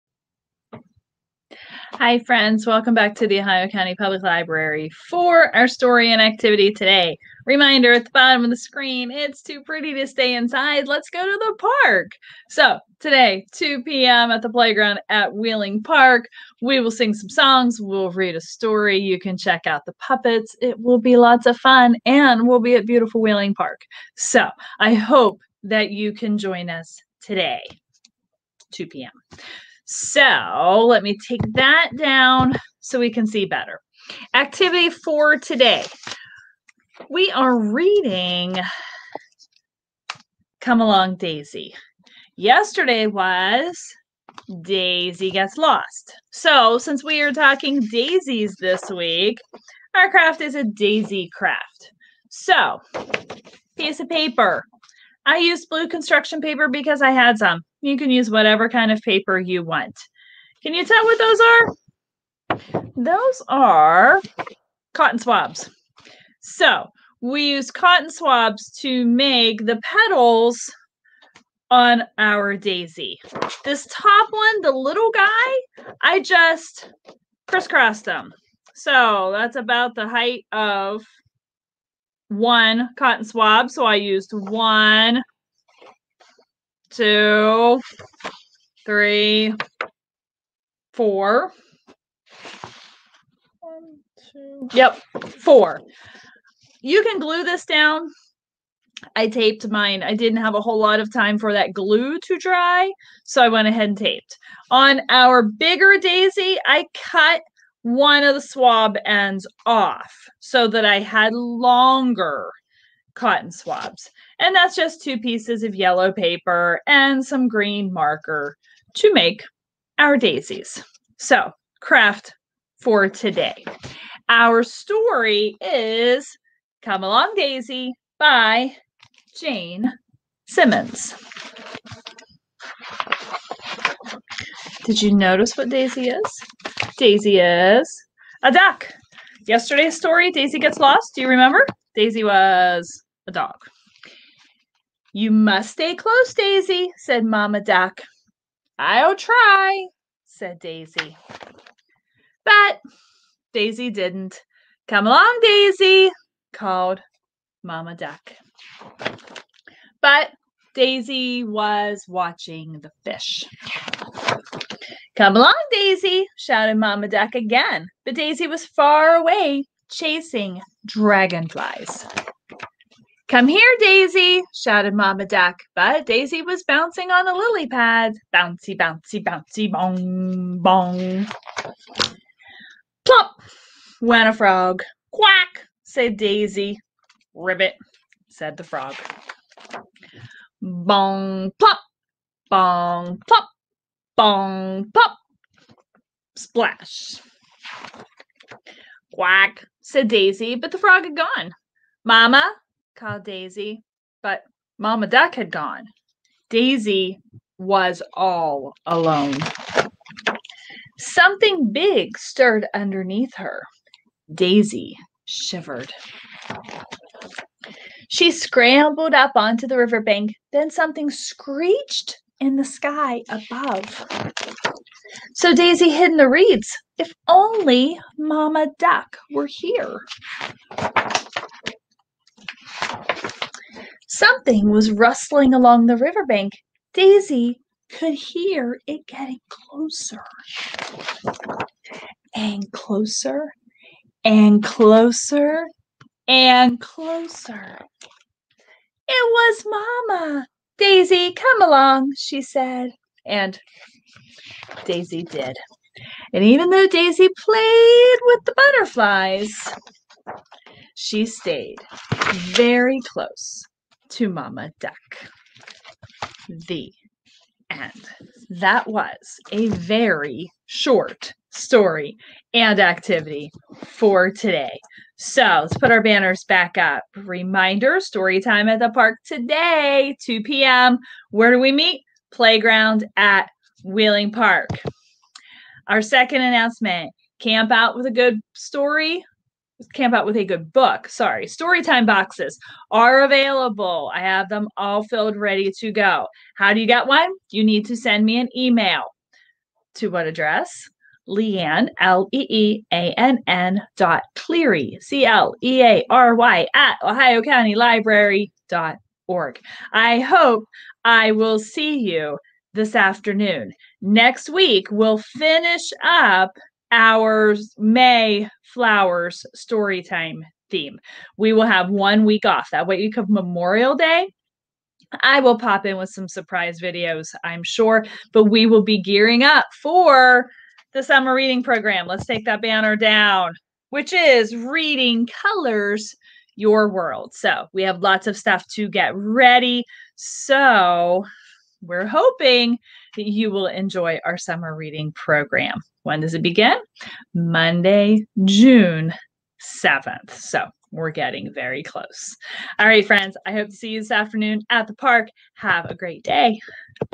Story and Activity
Today we will read Come Along Daisy and make a daisy picture.